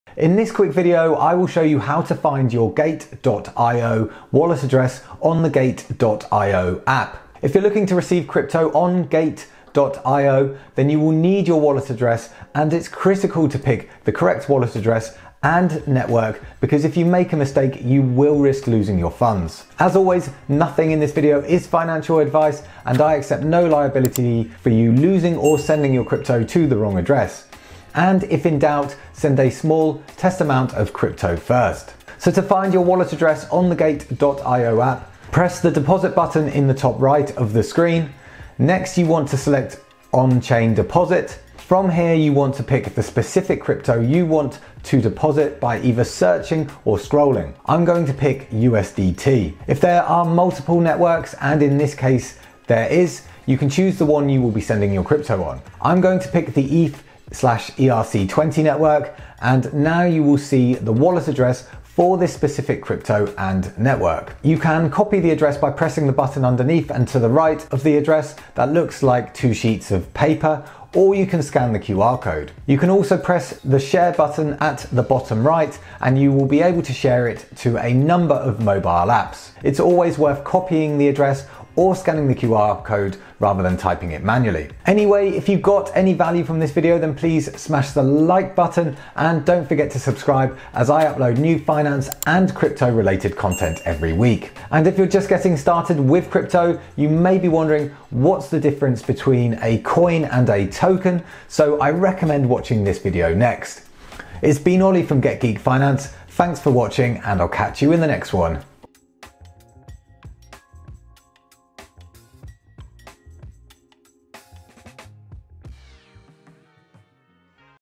HOW TO FIND YOUR GATE.IO WALLET ADDRESS ON GATE.IO APP (2026 Tutorial)
In this quick video i will show you how to find your Gate.IO wallet address on the Gate.IO Mobile App. If you're planning to send crypto to Gate.IO, then you will need your wallet address and its critical to pick the correct wallet address and network so in this video i explain how to find this information.

*** CRYPTOCURENCY EXCHANGES I USE (Ad)